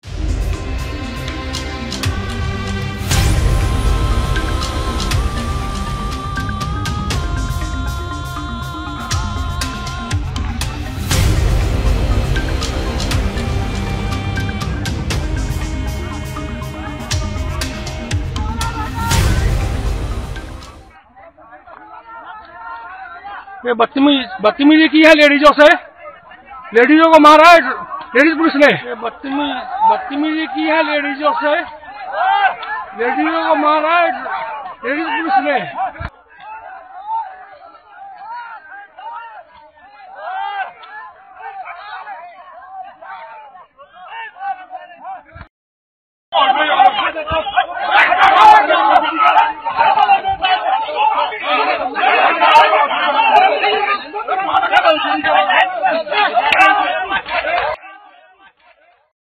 आगरा में भूमि अधिग्रहण से पीड़ित किसानों पर लाठीचार्ज ll Jantar Mantar Live
आगरा में 22 महीने से धरना दे रहे इनर रिंग रोड के भूमि अधिग्रहण से पीड़ित किसानों पर लाठीचार्ज एवं प्रशासन का अत्याचार हुआ एवं किसानों को गिरफ्तार करके थाना ताजगंज में ले जाया गया जहां पर 11 अक्टूबर 2018 की रात्रि को 11:00 बजे तक सभी किसानों के साथ प्रशासन से वार्ता करके किसानों को रिहा कराया गया है भारतीय किसान यूनियन प्रशासन को आगाह करती है कि किसानों पर अत्याचार एवं किसानों का शोषण बंद किया जाए नहीं तो भारतीय किसान यूनियन एक बड़ा आंदोलन करने के लिए बाध्य होगी...